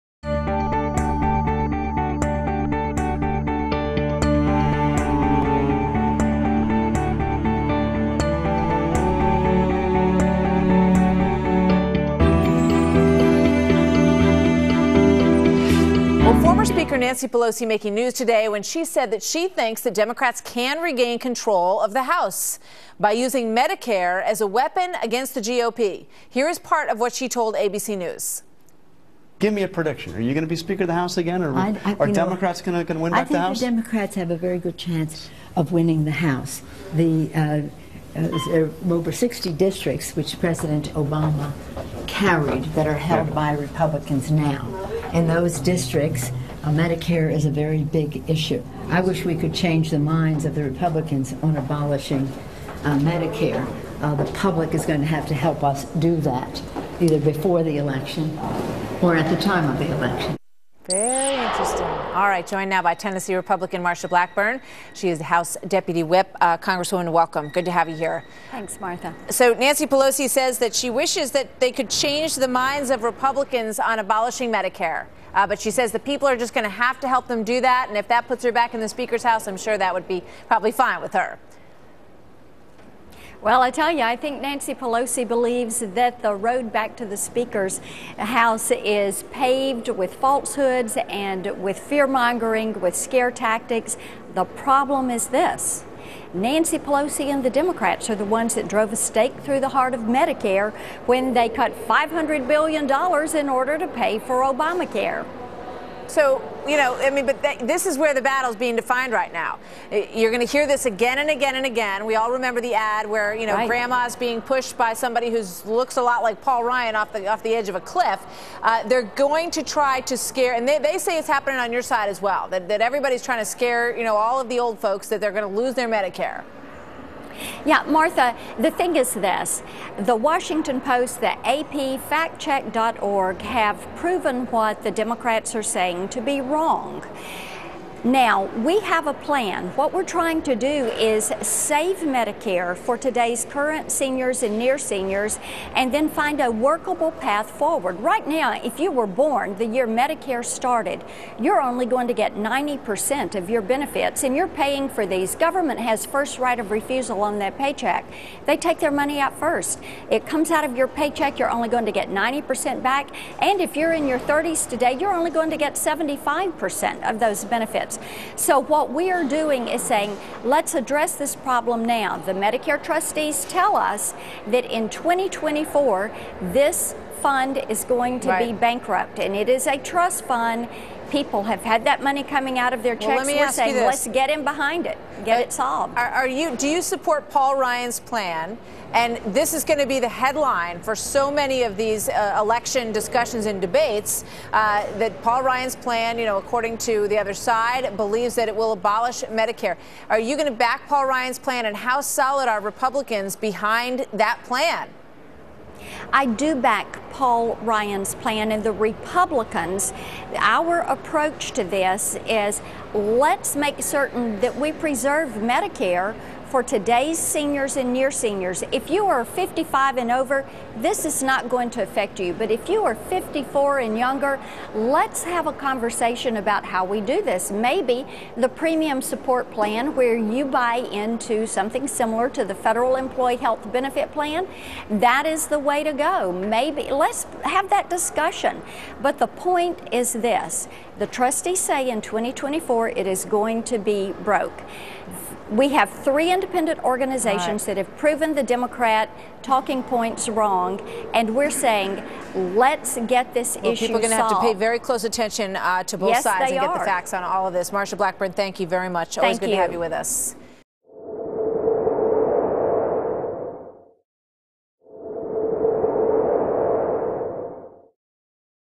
HEALTH CARE: "Mediscare"
Three independent sources have called "Mediscare" tactics false.  Marsha takes the fearmongers on.